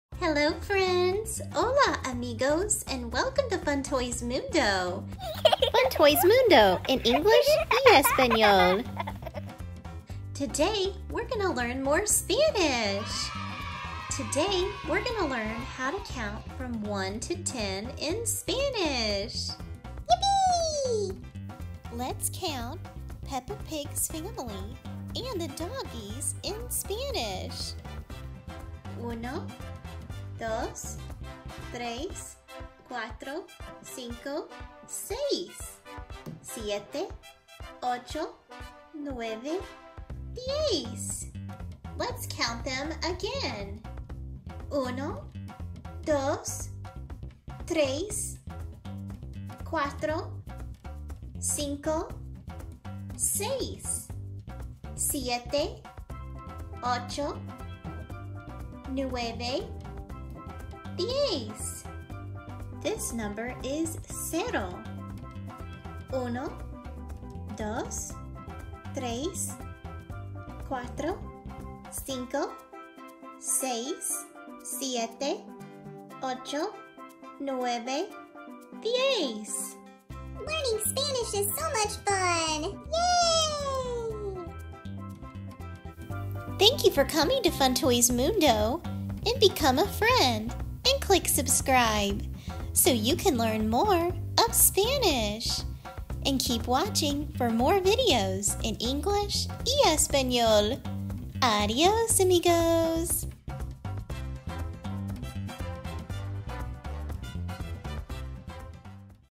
Learn Spanish Numbers for Beginners
Learn Spanish with Fun Toys! Our Spanish Lessons are: “Spanish for Beginners” geared toward kids, however you can learn Spanish at any age! We make learning FUN with toys🤗 Check out the Fun Toys Mundo channel for more Spanish lessons and topics.

🎼🎶Music From...